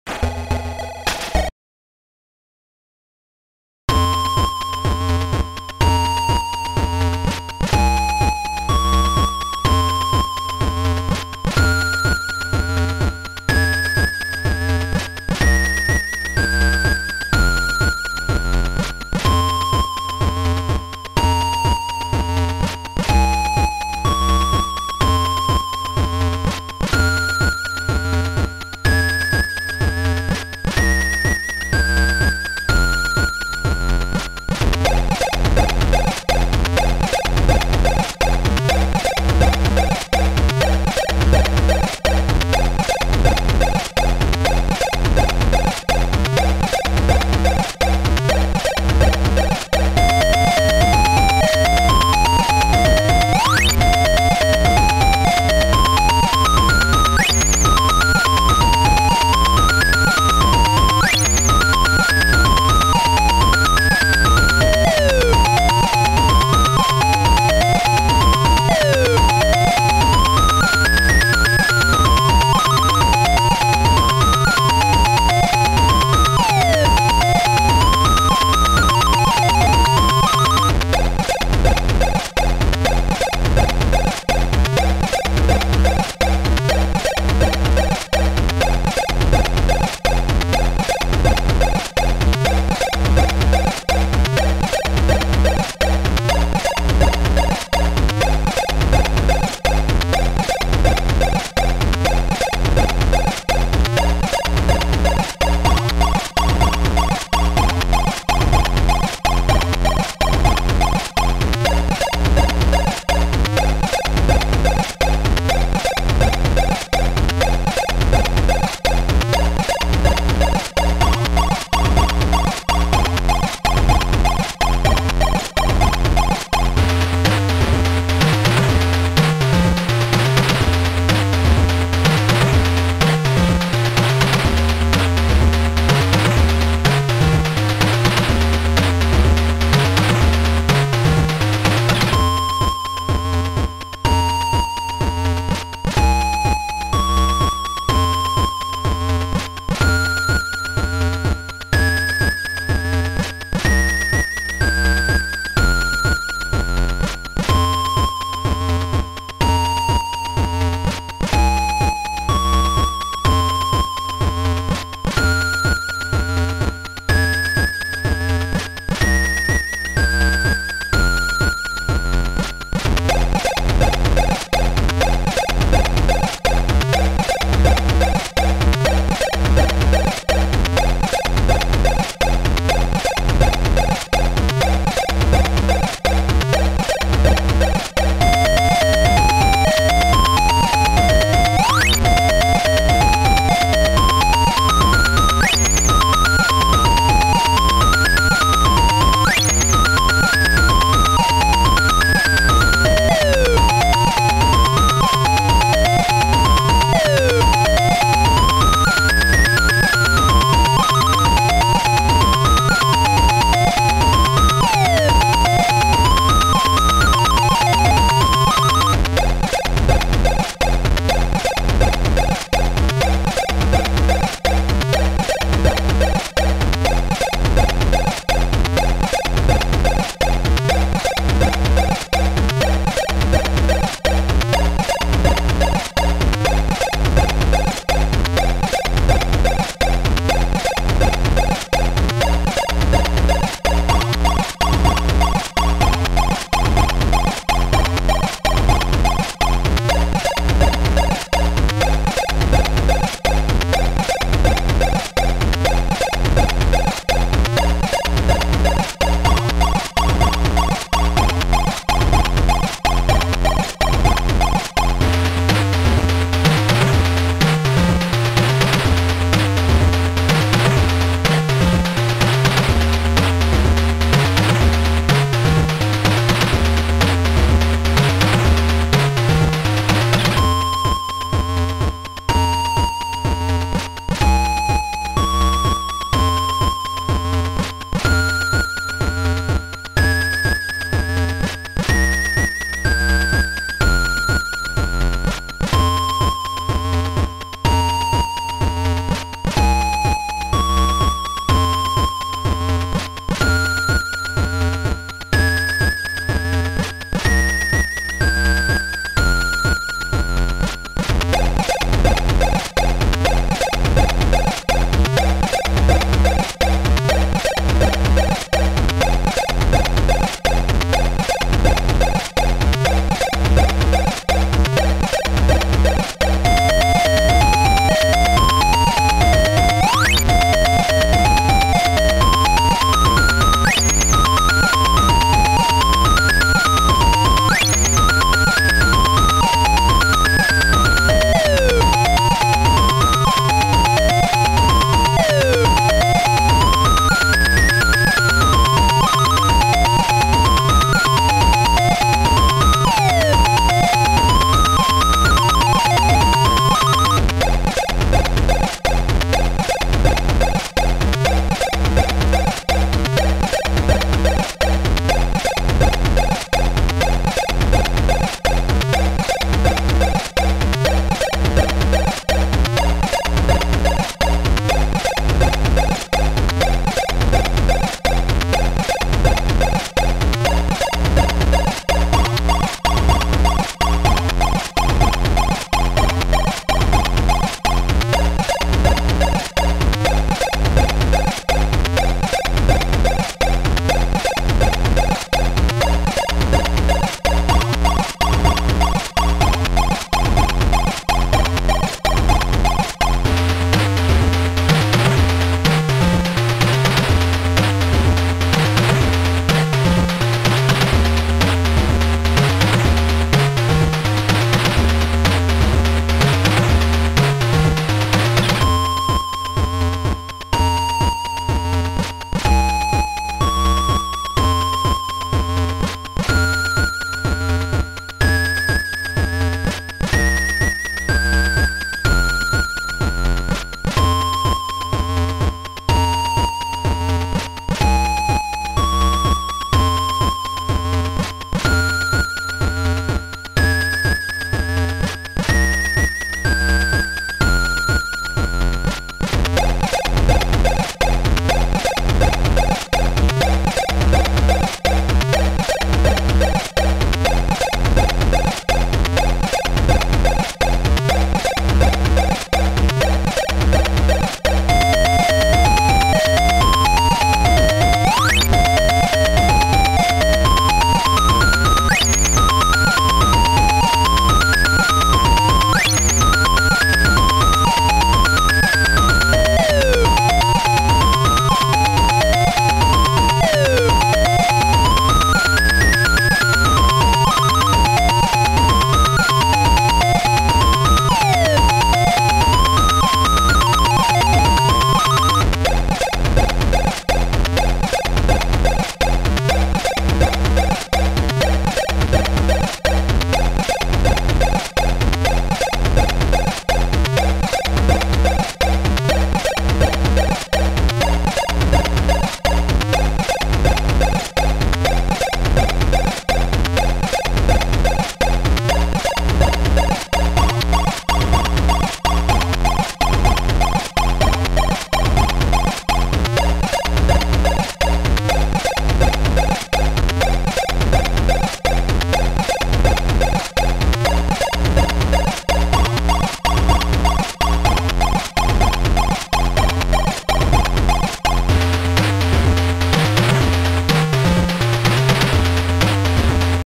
TCB screen - The Whattaheck Demo (Atari ST)
This is the TCB screen from the Whattaheck Demo.  Coded by TCB, music by Mad Max.